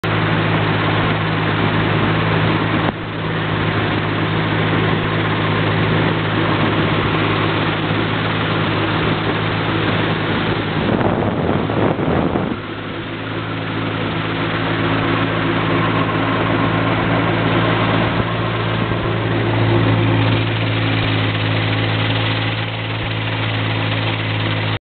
World's Fastest Paddlewheel Boat (Part 3)
St. Andrew Bay / North Bay Panama City, Fl.
Clemson Mechanical Engineer, Dan, at helm.
Speed - Fast Idle
30May11